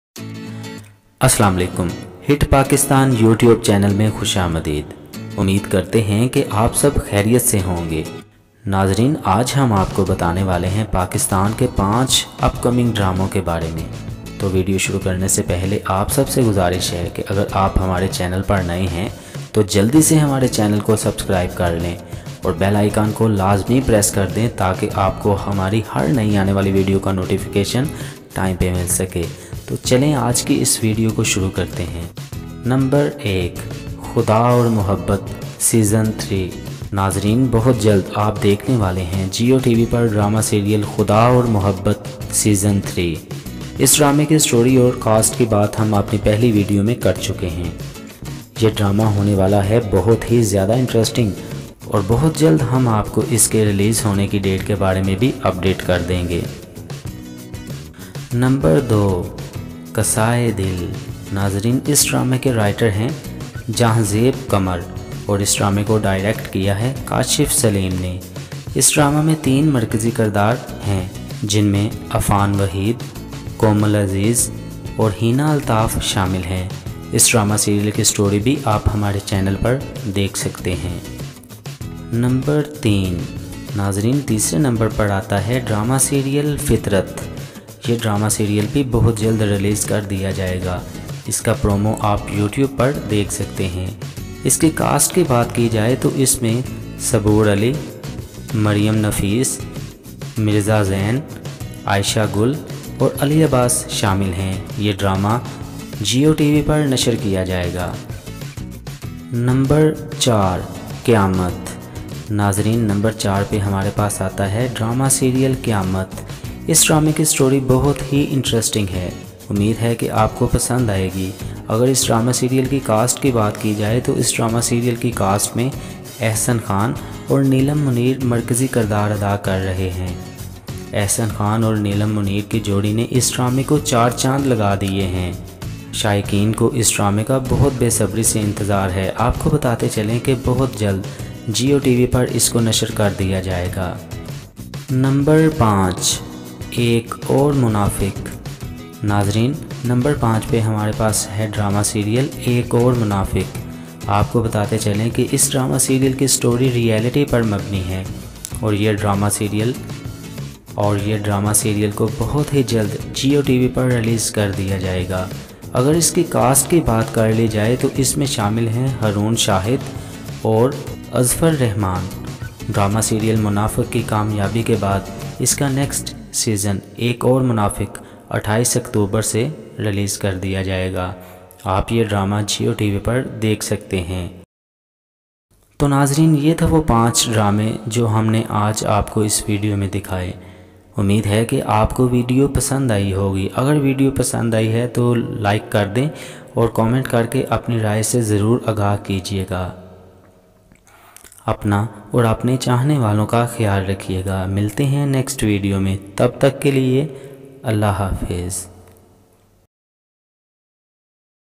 Top 5 pakistani upcoming dramas 2020/2021 || Hit Pakistan
In this video i tell you about top 5 pakistani upcoming dramas 2020 / 2021